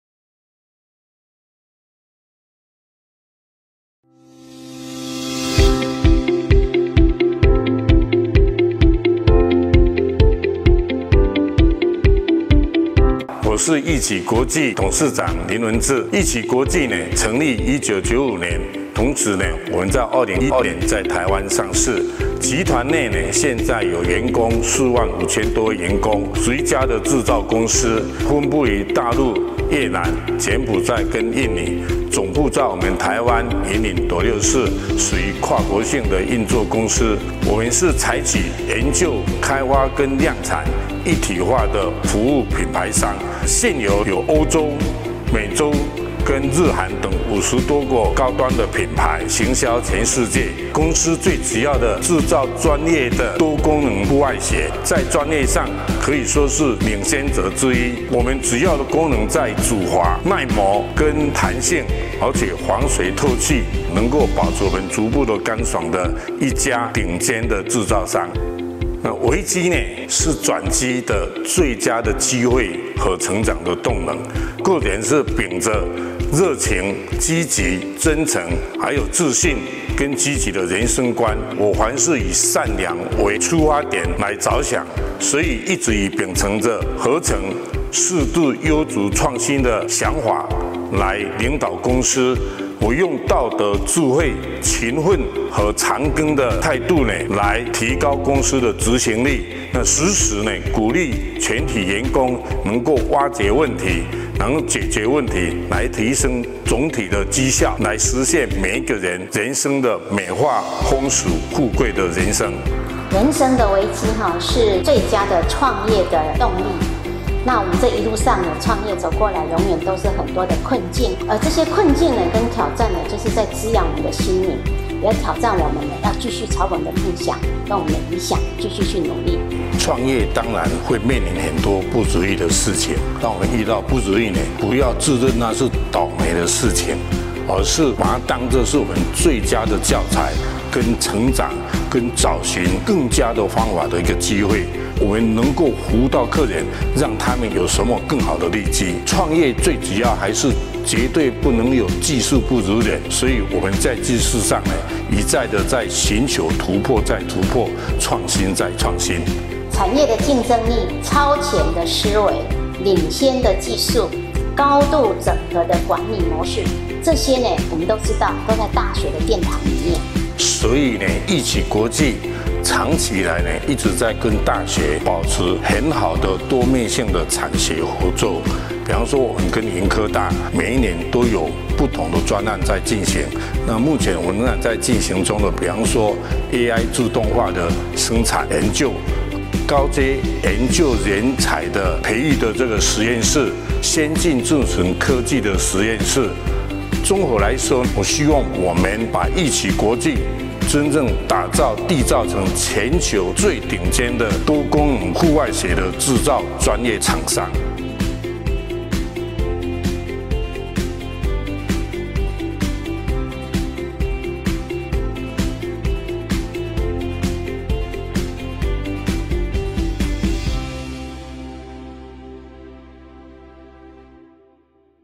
雲科大【產學連結】鈺齊國際公司與雲科大產學共舞1120519
產學合作優質企業「鈺齊國際股份有限公司」
採取研究、開發和量產一體化的服務品牌商，
專門製造專業的多功能戶外鞋，
並與雲科大保持著良好且多面性的產學合作~
一同了解"鈺齊國際"顛覆傳統產業思維~